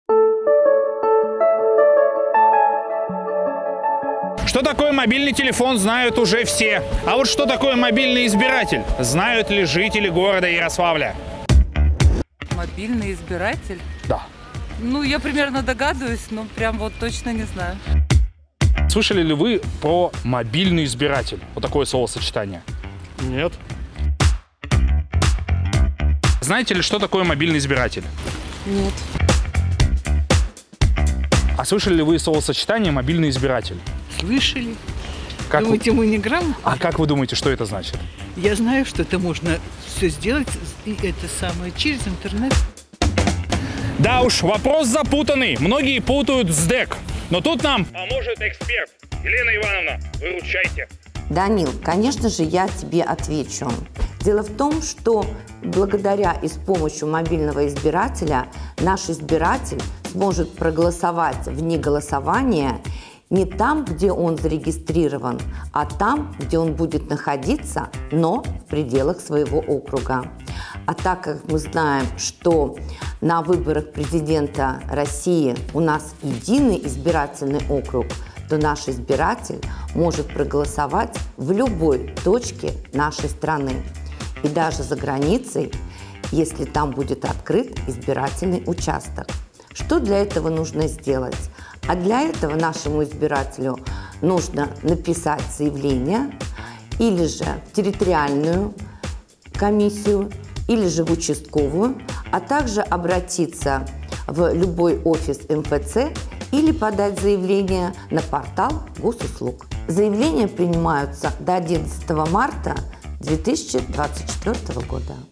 Мобильный избиратель
Выборы 2024/ Выпуск 4

Что такое Мобильный избиратель?
Такой вопрос мы задали жителям Ярославля.
И как оказалось, многие этого не знают.

Разбираемся с экспертом - председателем избирательной комиссии Ярославской области Еленой Новик.